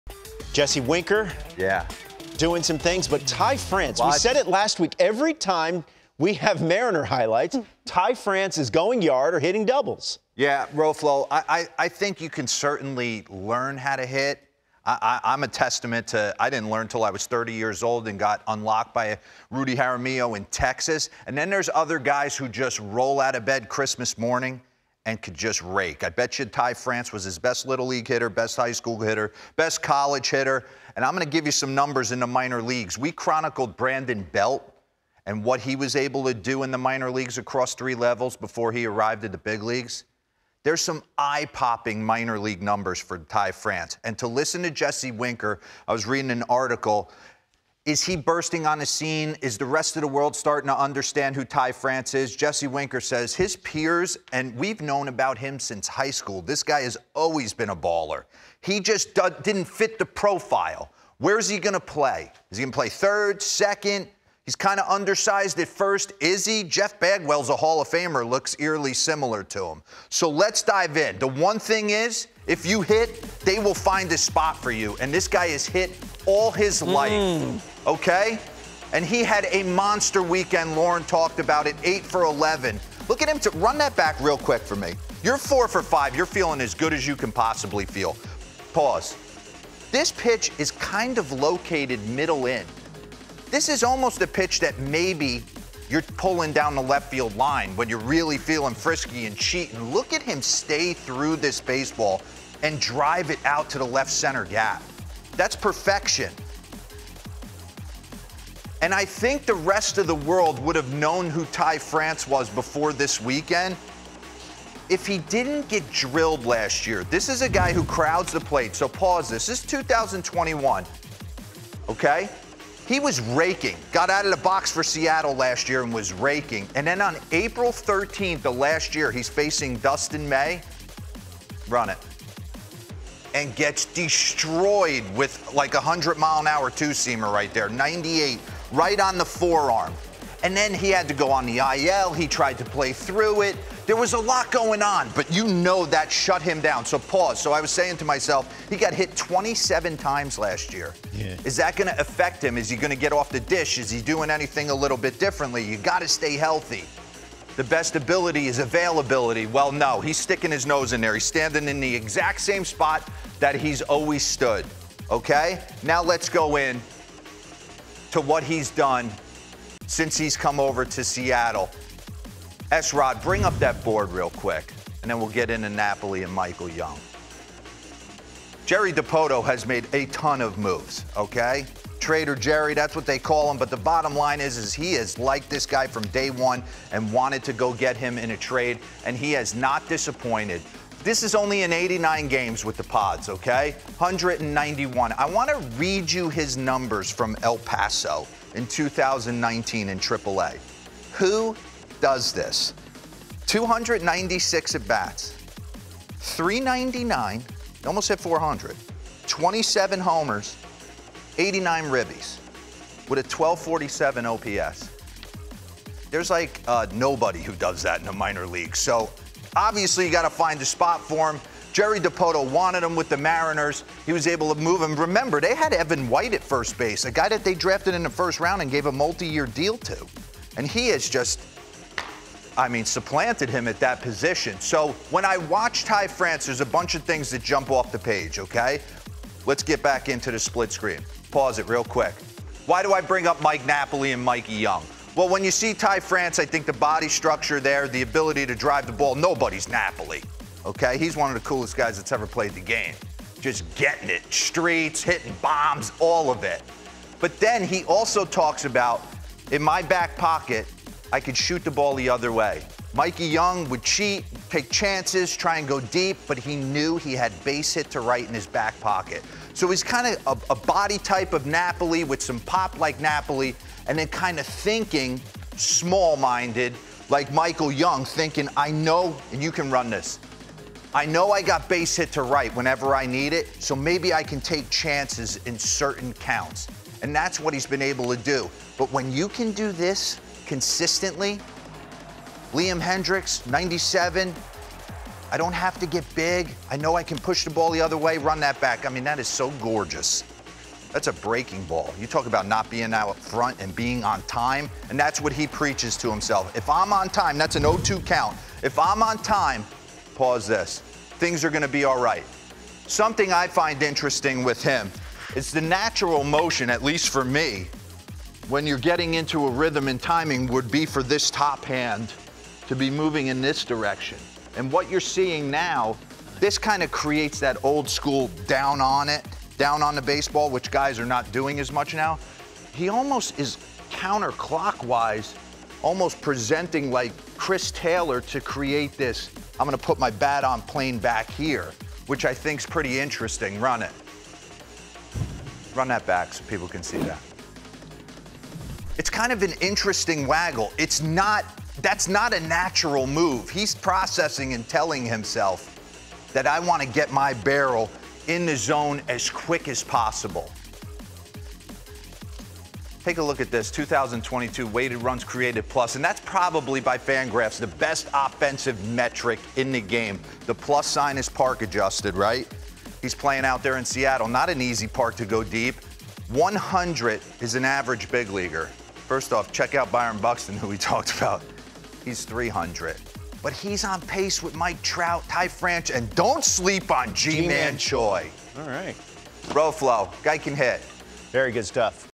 Ty France breakdown
Mark DeRosa heads to the skybox to breakdown Ty France, comparing him to Mike Napoli and Michael Young